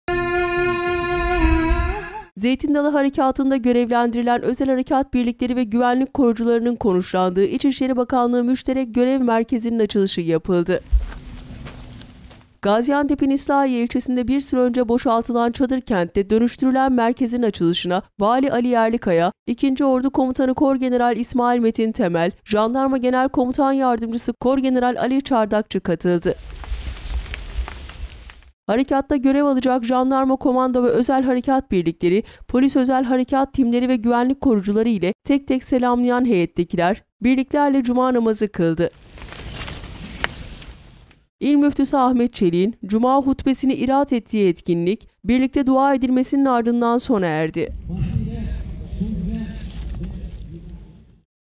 MÜŞTEREK GÖREV MERKEZİ’NDE CUMA NAMAZI
Zeytin Dalı Harekatı'nda görevlendirilen özel harekat birlikleri ve güvenlik korucularının konuşlandığı İçişleri Bakanlığı Müşterek Görev Merkezinin açılışı yapıldı.
Gaziantep'in İslahiye ilçesinde bir süre önce boşaltılan çadır kentten dönüştürülen merkezin açılışına Vali Ali Yerlikaya, 2. Ordu Komutanı Korgeneral İsmail Metin Temel, Jandarma Genel Komutan Yardımcısı Korgeneral Ali Çardakçı katıldı.
Harekatta görev alacak jandarma komando ve özel harekat birlikleri, polis özel harekat timleri ve güvenlik korucuları ile tek tek selamlayan heyettekiler, birliklerle cuma namazı kıldı.
İl Müftüsü Ahmet Çelik'in cuma hutbesini irat ettiği etkinlik, birlikte dua edilmesinin ardından sona erdi.